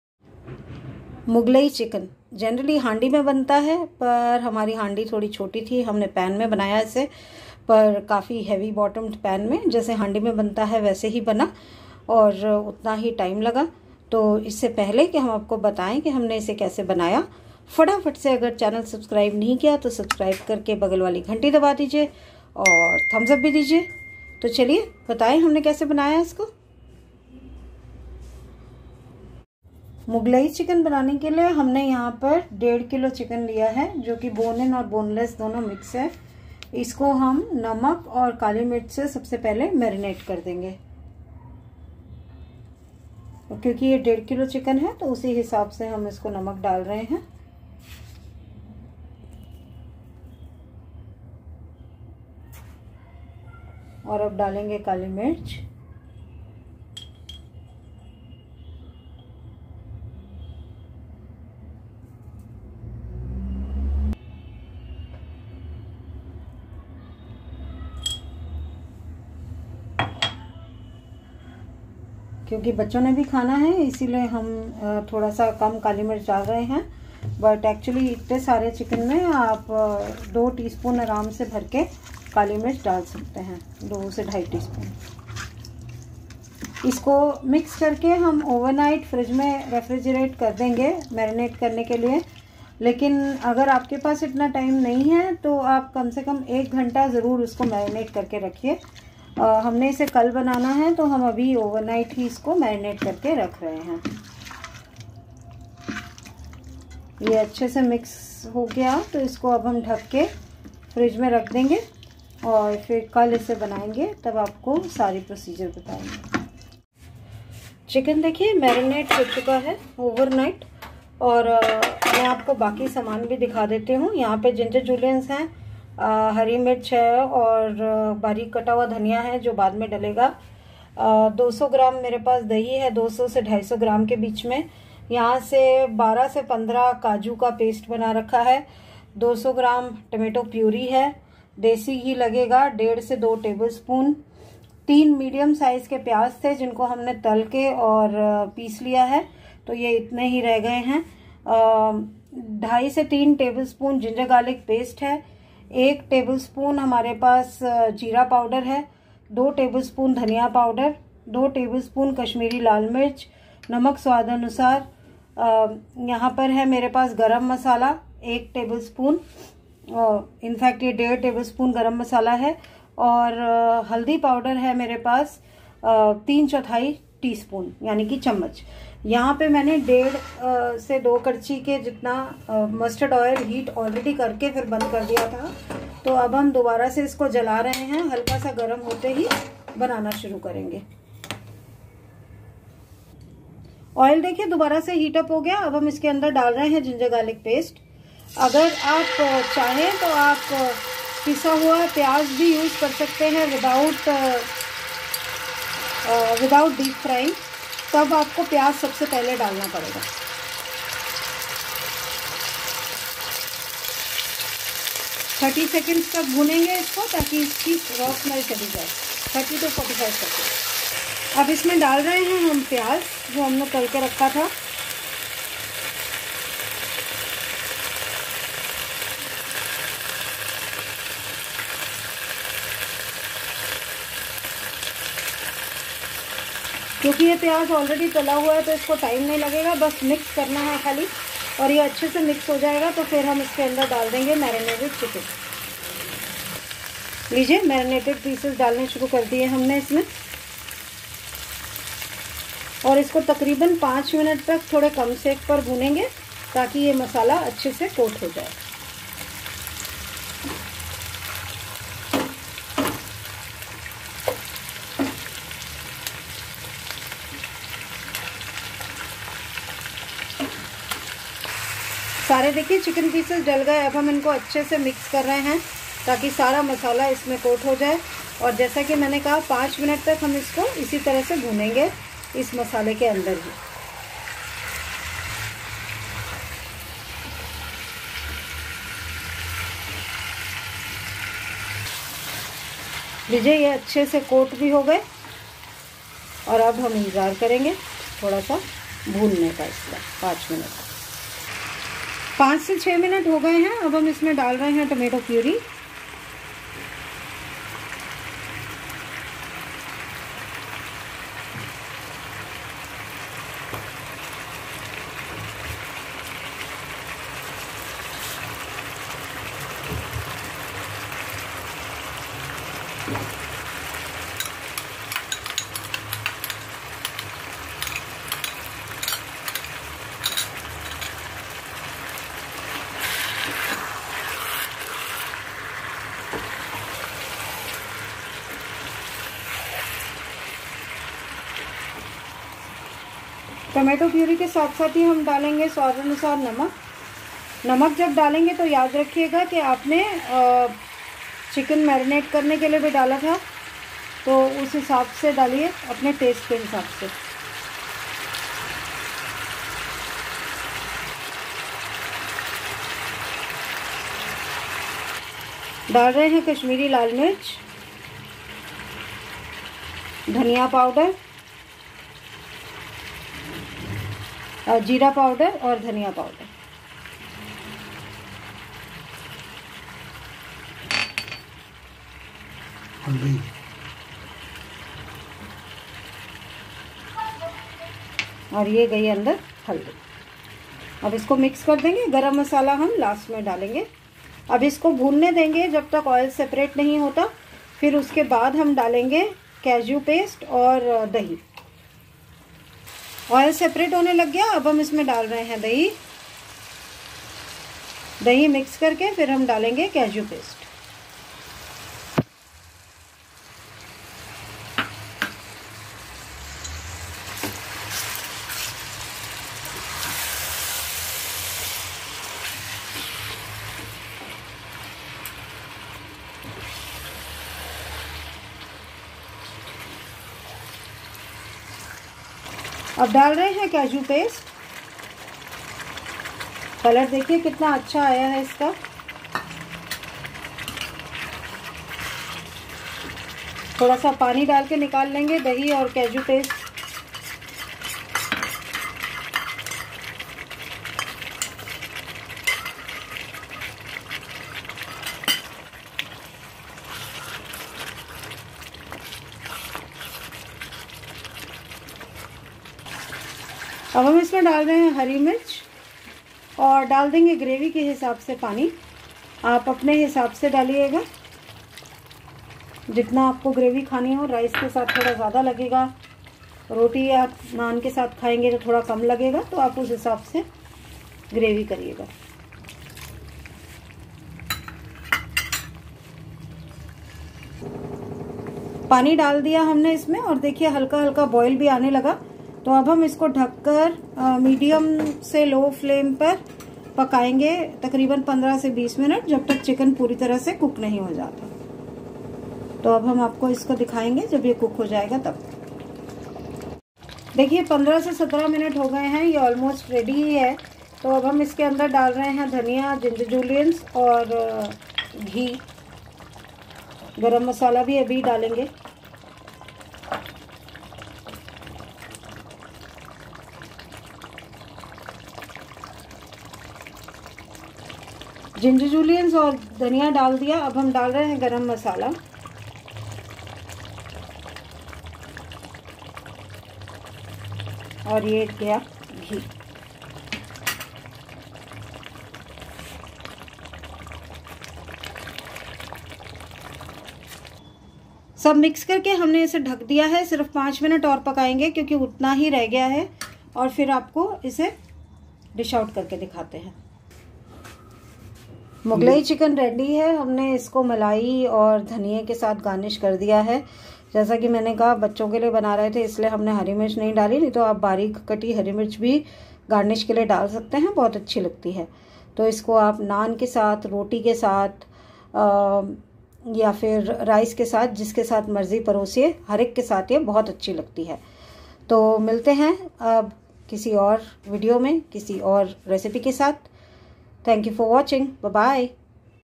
Mughlai Handi Chicken
Ingredients:
1.5 Kg Chicken (we used bone in, and boneless both) marinated overnight or at least for an hr with salt and 2 tsp black pepper powder
1 tbsp Ginger juliens
2-3 tbsp chopped fresh coriander
2-3 whole green chillies
200-250 gms curd
Paste of 12-15 cashews
2 tbsp fresh cream or malai (optional - for garnishing)
200 gms tomato puree
3 medium sized onions sliced, fried and made to a paste, or puree of 3 onions without frying
2.5 - 3 tbsp ginger garlic paste
1-1.5 tbsp ghee
1.5 - 2 ladelful (karchi) Mustard oil
1 tbsp cumin powder
2 tbsp coriander powder
2 tbsp kashmiri red chilli powder
1.5 tbsp garam masala
3/4th tsp turmeric powder

Method:
Heat mustard oil in a heavy bottomed pan or handi. If using raw onion puree, add at this time and sate till slightly brown in color, and then add ginger garlic paste. If using fried onion, then first add ginger garlic paste, cook till raw smell goes away, and then add fried onion paste and mix.
Add marinated chicken pieces, cook on low flame for 5 minutes turning occasionally so the masala coats the chicken pieces nicely, and the chicken is roasted.
After 5 minutes of cooking, add tomato puree, salt to taste, kashmiri red chilli powder, turmeric powder, coriander and cumin powder. Mix well and cook till oil separates.
Once oil separates, add curd, mix, then add cashew paste. Cook till oil separates then add green chillies,  sufficient water for gravy. Let a boil come, then cover for 15-17 mins till chicken is done.
Add fresh coriander,  ginger juliens, ghee and garam masala. Mix, cover and cook for 5 more mins. Dish out, garnish with fresh cream/ malai, fresh coriander and finely chopped green chillies (optional). Serve with hot boiled rice/ nan/ tandoori or khameeri roti.